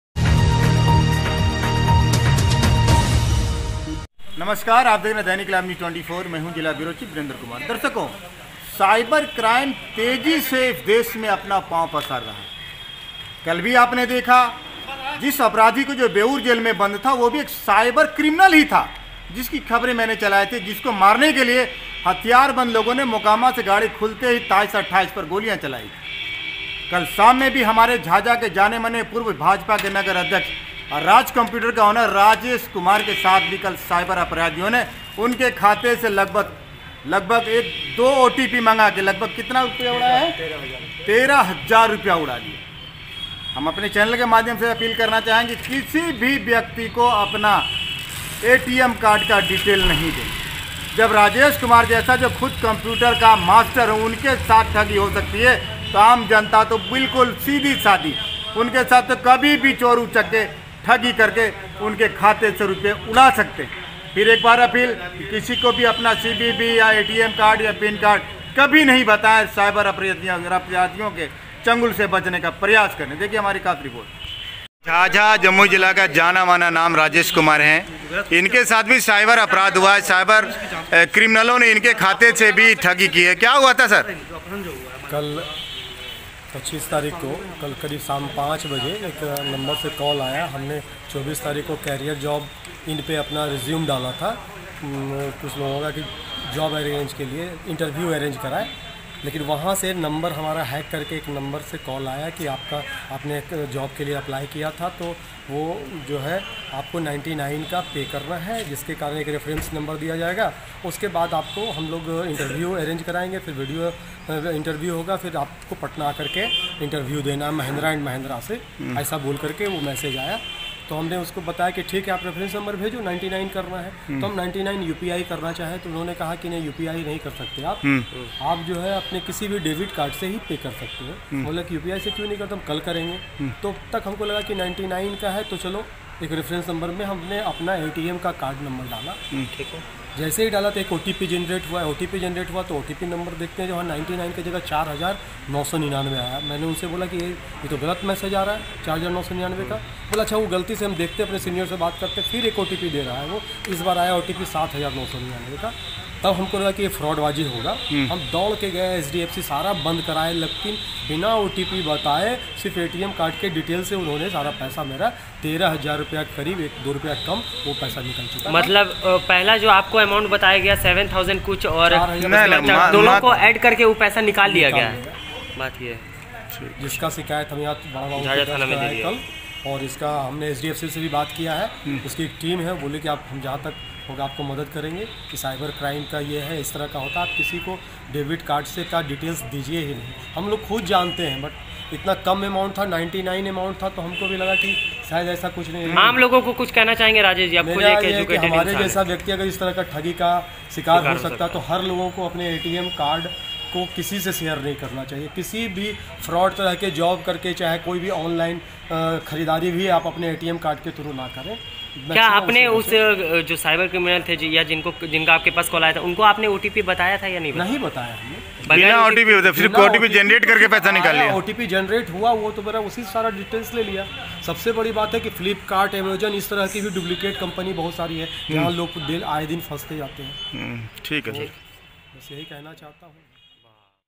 पूर्व भाजपा नगर अध्यक्ष, राजेश कुमार के साथ हुआ साइबर क्राइम।
har khabbar sachaai ke saath

       THANKS